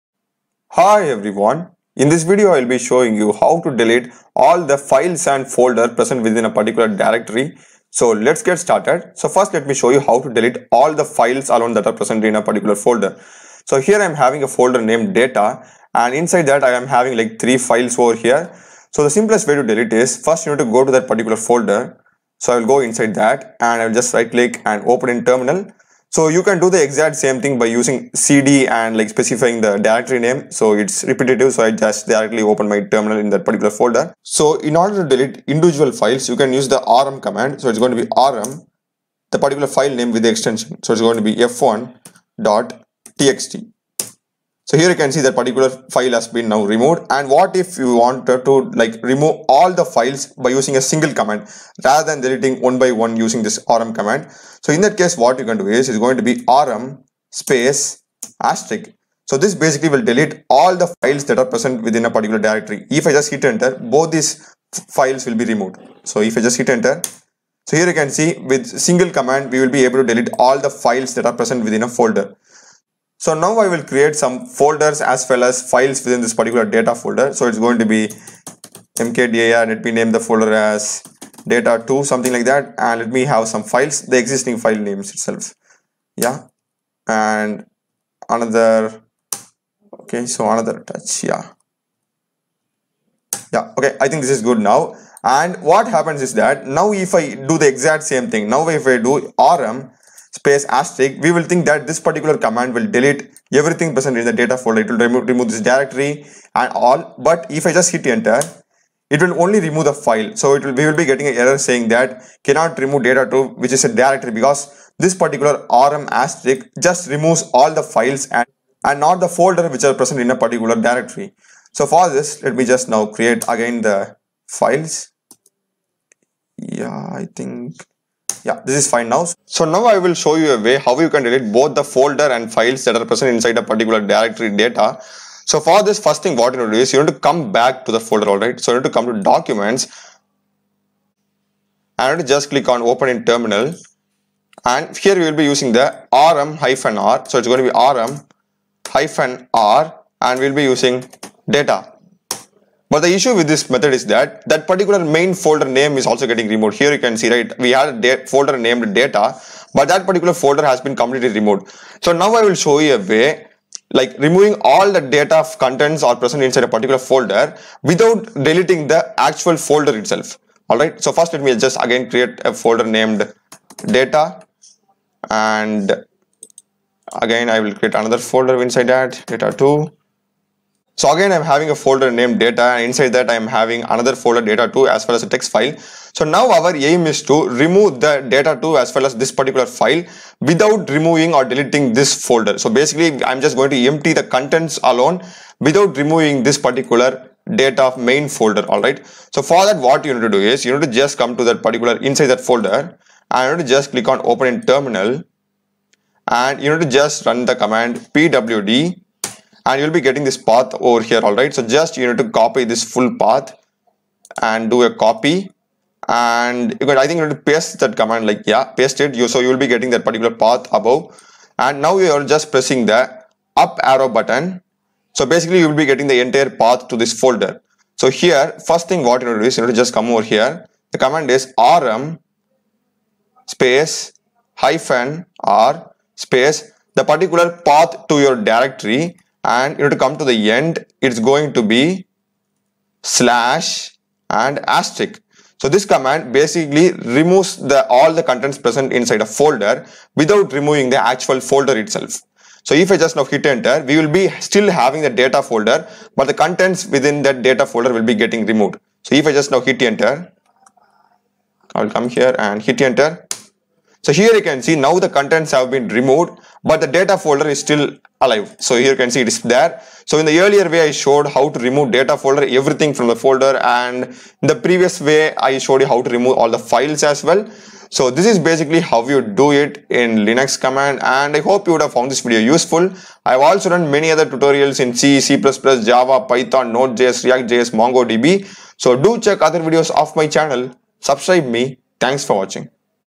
Linux command to delete all files in a folder or directory using 2 ways
How to delete all files and folders in a directory using linux command is shown